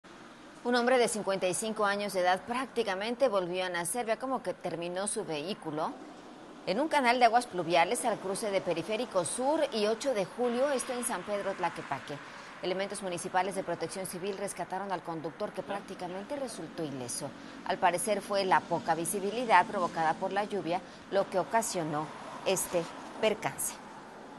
Vuelca y cae automóvil en un canal pluvial | Imagen GDL con Fabiola Anaya
Un auto cayó en un profundo canal de aguas pluviales en San Pedro Tlaquepaque, el conductor resultó ileso.